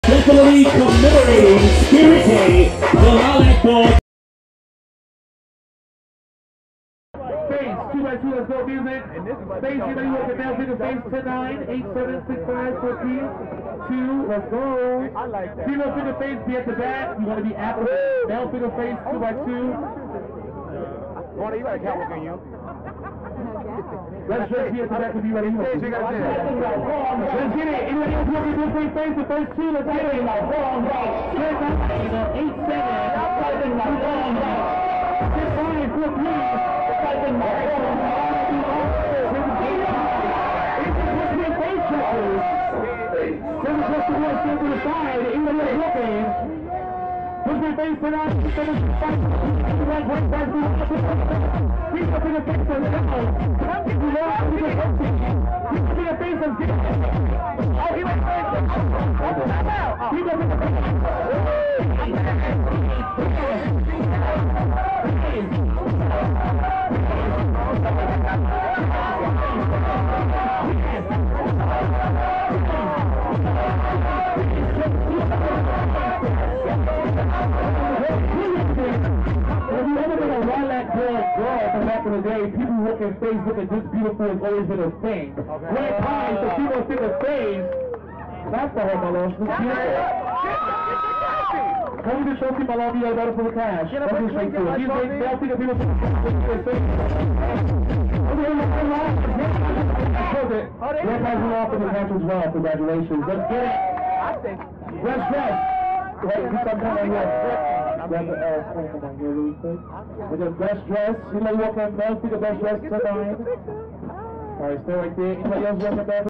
OTA Face @ The Lilac Ball 2022
An Event Commemorating Spirit Day. We're giving awareness to suicide and bullying.

Destination Tomorrow Presents:
The Lilac Ball

MC of the Night: The Iconic Snookie Juicy

Categories:
Every category is "just bring it" with the color purple. Be Creative!
{All purple, a little bit of purple, a touch of purple is all permitted.}
OTA Performance
OTA Runway
OTA Best Dressed
OTA Face
OTA Realness